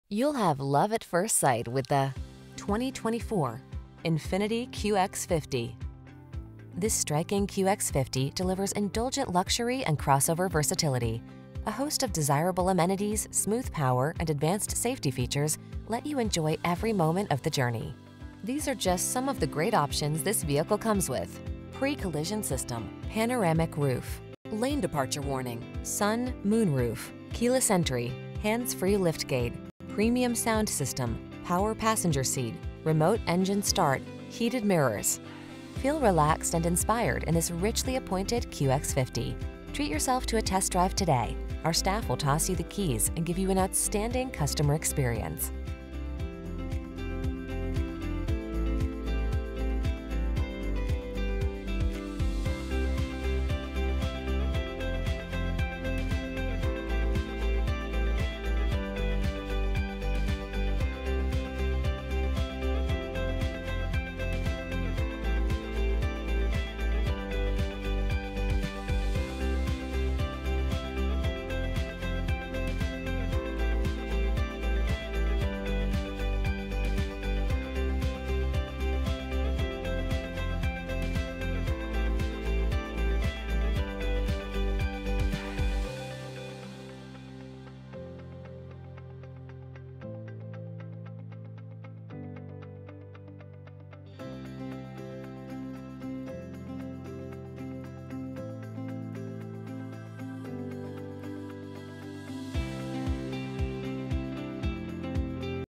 2024 INFINITI QX50 Cerritos, Los Angeles, Anaheim, Huntington Beach, Long Beach, CA RF100874
3PCAJ5BA7RF100874 Graphite Shadow New 2024 INFINITI QX50 LUXE available in Cerritos, California at Cerritos Nissan. Servicing the Los Angeles, Anaheim, Huntington Beach, Long Beach, CA area.

Cerritos Nissan
18707 E. Studebaker Rd

2024 Graphite Shadow INFINITI QX50 LUXE FWD CVT I423/29 City/Highway MPG